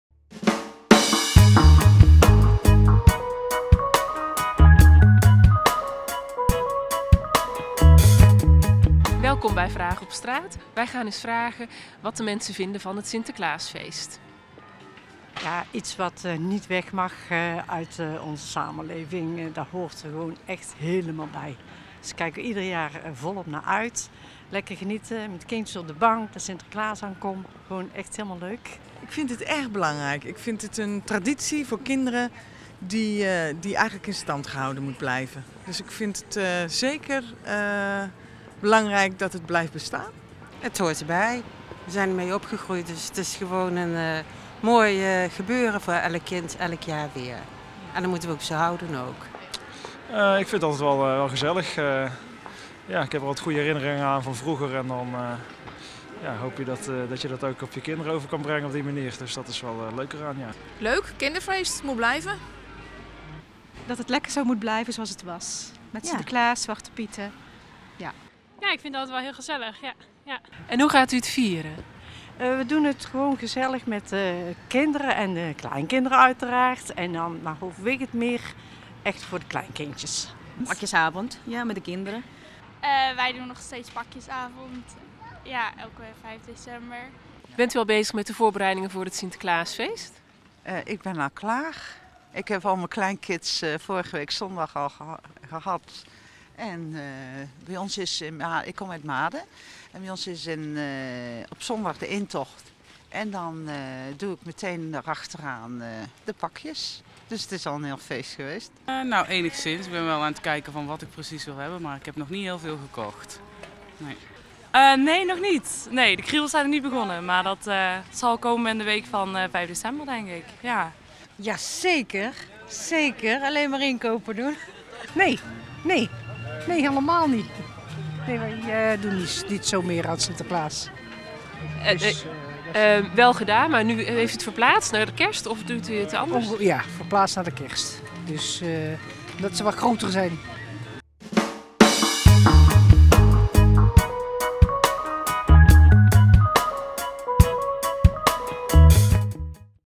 Vraag op Straat: Wat vindt u van het Sinterklaasfeest?
In deze editie van Vraag op Straat vroegen we Breda: 'Wat vindt u van het Sinterklaasfeest?'.